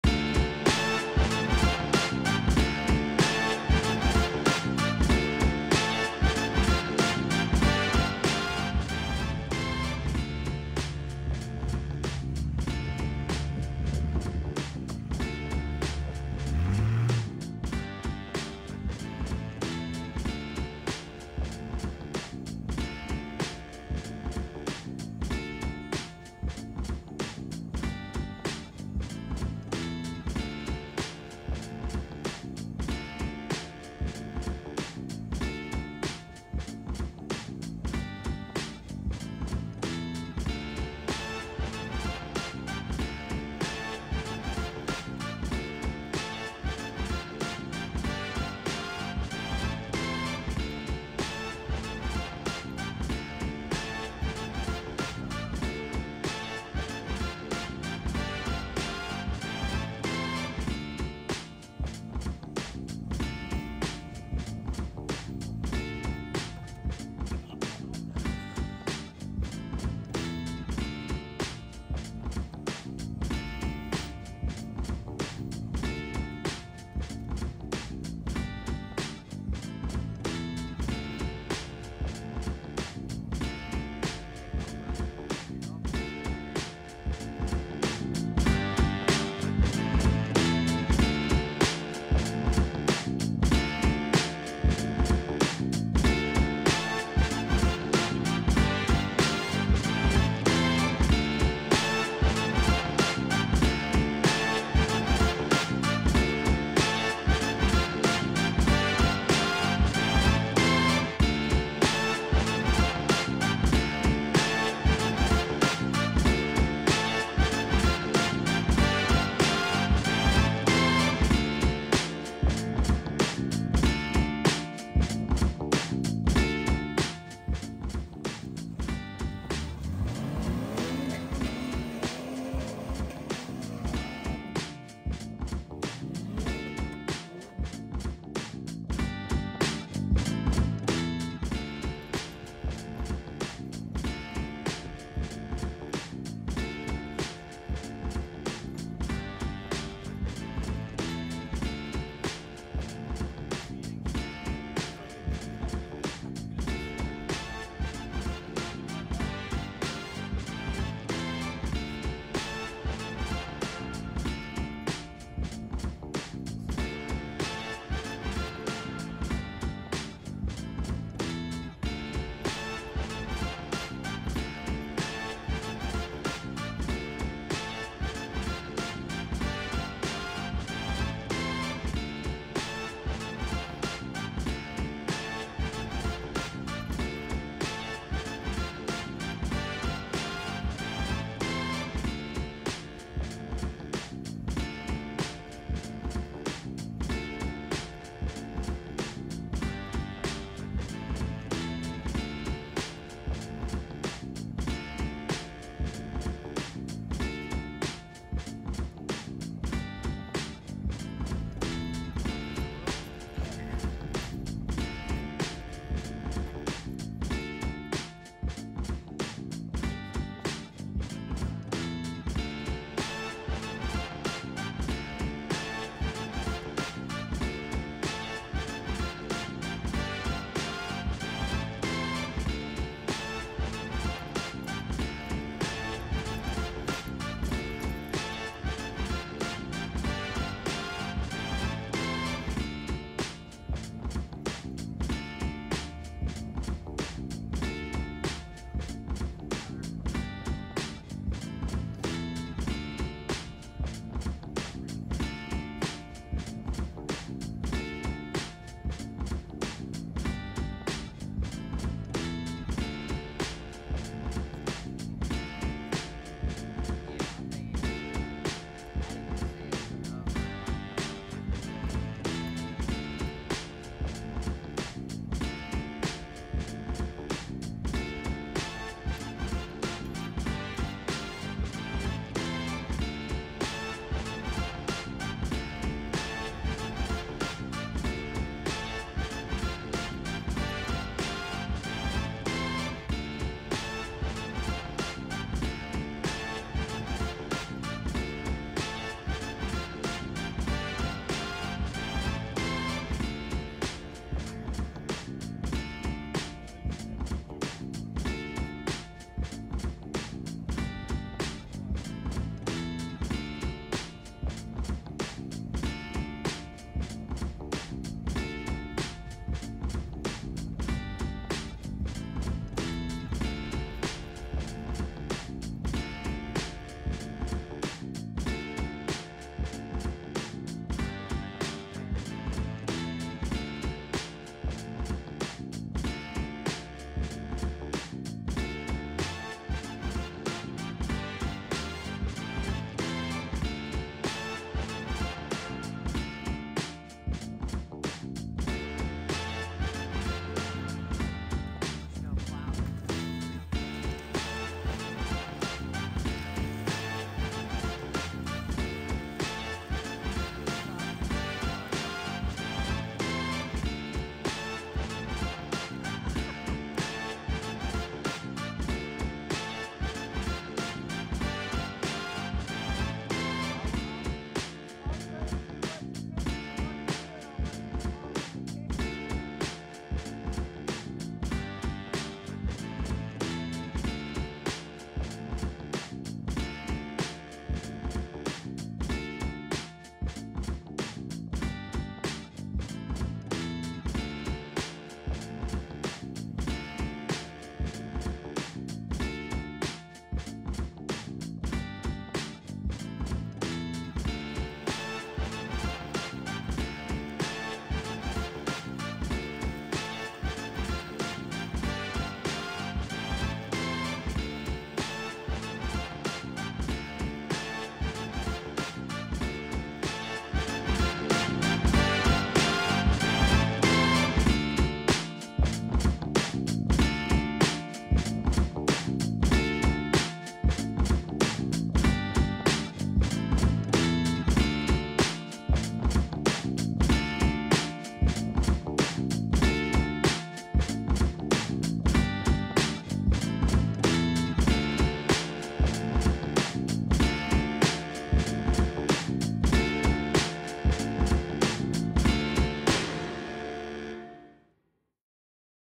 Trailing in the Jeep
Braving the frigid temperatures and wheelin our jeeps